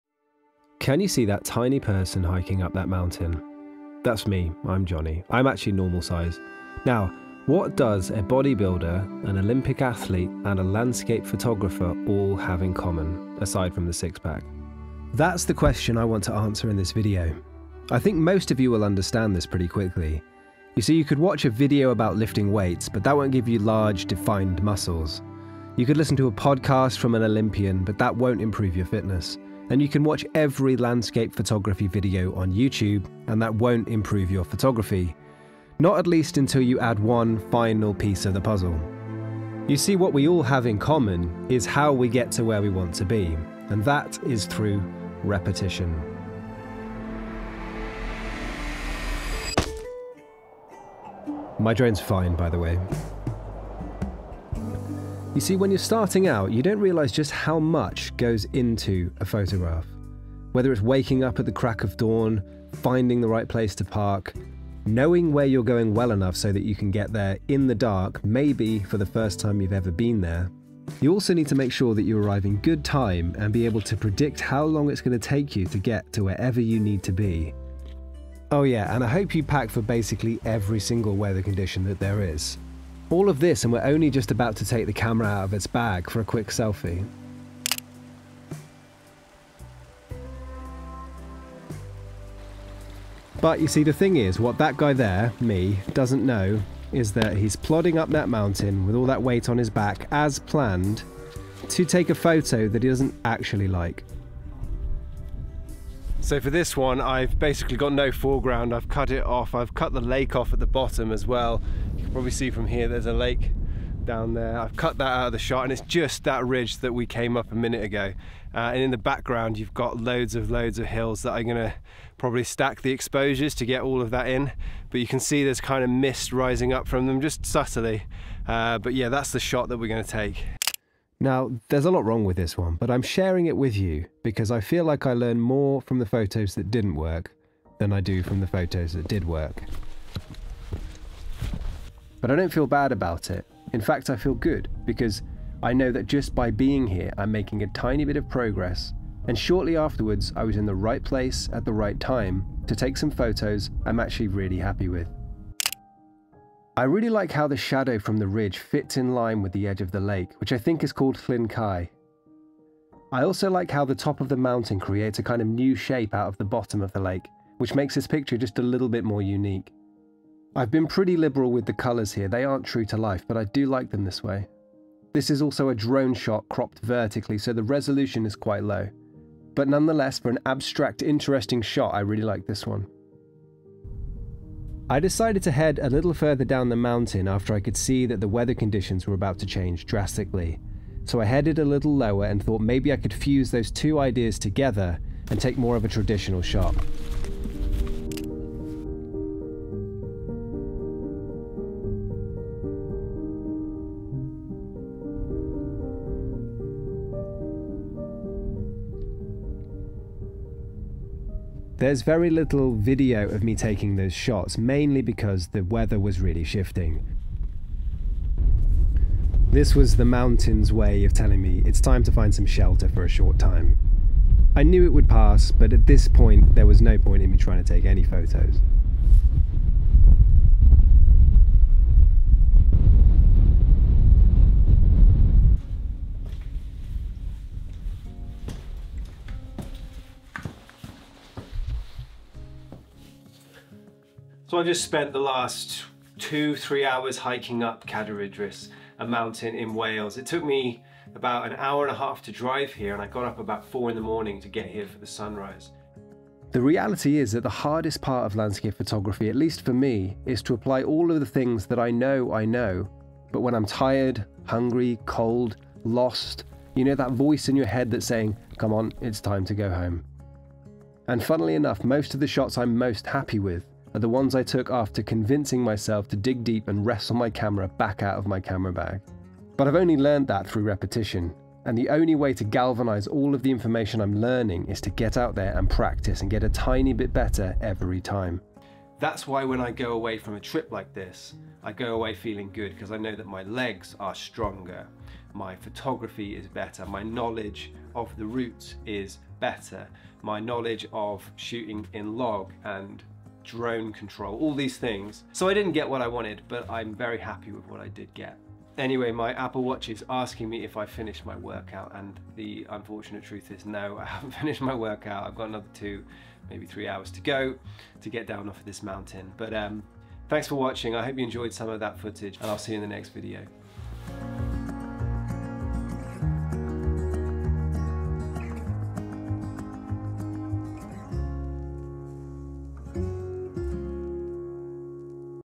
The SIMPLEST Photography Lesson of All
If you're looking for landscape photography tips that's great! There are so many amazing videos out there teaching all the theory of photography. But at some point you need to put it into practice, over and over again. In this video I hike cadair idris in southern snowdonia national park which has become one of my favourite photography locations.

🏞 LANDSCAPE PHOTOGRAPHY GEAR 👇
⚡️CAMERA - Sony A7IV
⚡️ MY MAIN TRIPOD
⚡️ MY WIDE LENS
⚡️ MY 35mm LENS